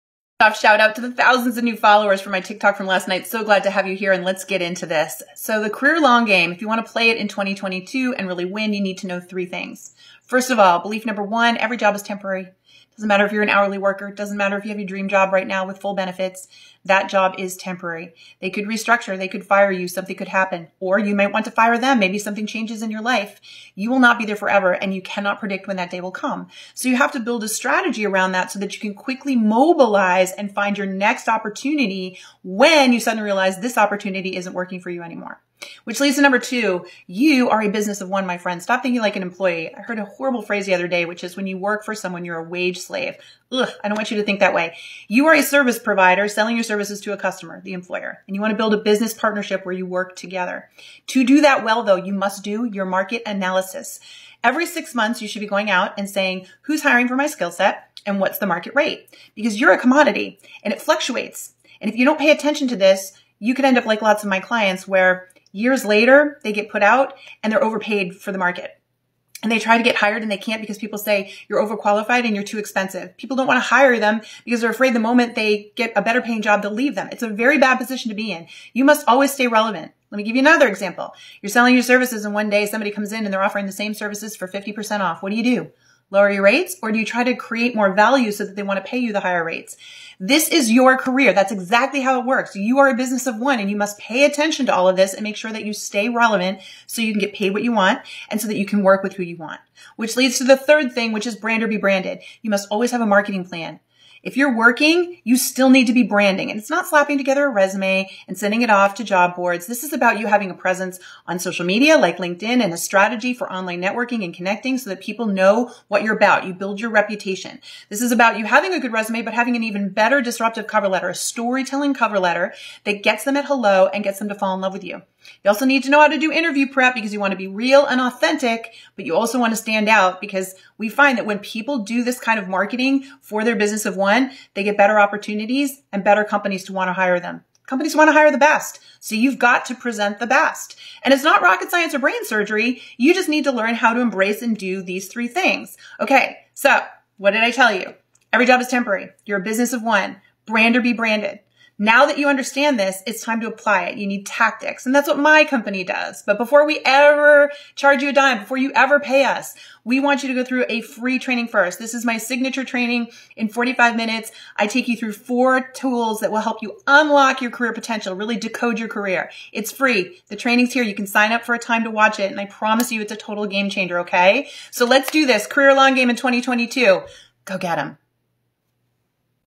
How To Play The Career Long-Game In 2022
Learn how to optimize your resume & LinkedIn profile with our free training

If you want to play the career long-game in 2022 you need to know three things:

1. Every job is temporary
2. You are a business-of-one
3. Brand or be branded

Want great career advice in your feed every day? Follow JT & Work It Daily on TikTok!

Check out our library of free career and job search resources

More from Work It Daily